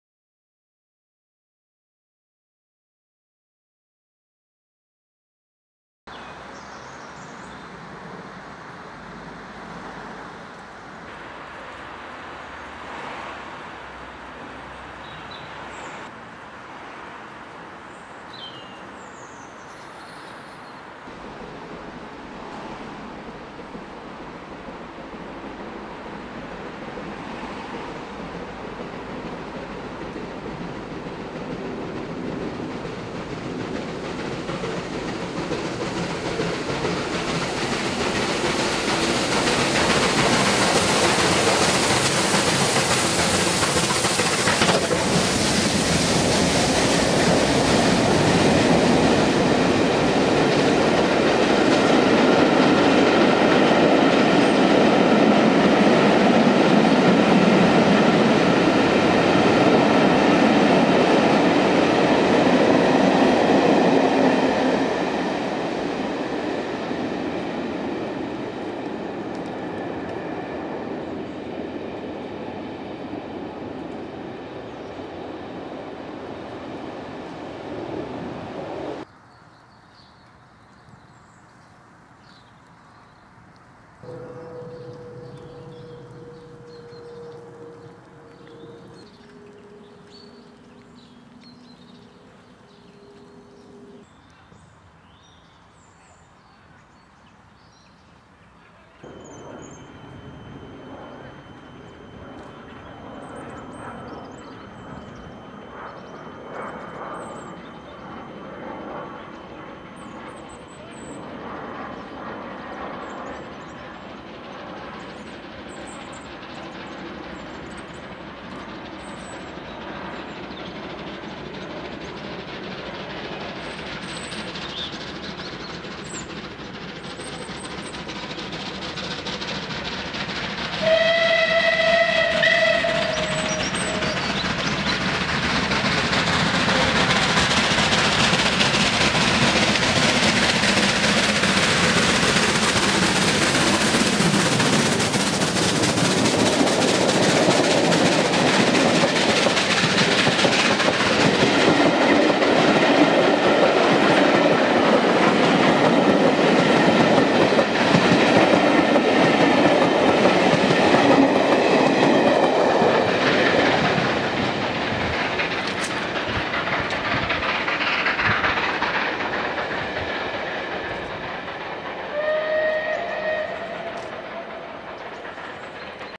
MN no.35028 'Clan Line' with a Surrey Hills VSOE - Saturday 9th February 2013
MN no.35028 'Clan Line' is seen in two clips on my first video of 2013 with a VSOE Surrey Hills Luncheon tour London Victoria-Staines-Guildford-Redhill-London Victoria. First is from the bridge over the line off Priory Court, overlooking the exit of the 852 yard Guildford Chalk Tunnel as Clan Line runs towards Shalford Junction. We then see the departure from the Shalford water stop and the start of the climb of Gomshall Bank as Clan Line passes over East Shalford Lane level crossing at Chilworth 09/02/13.

Photo...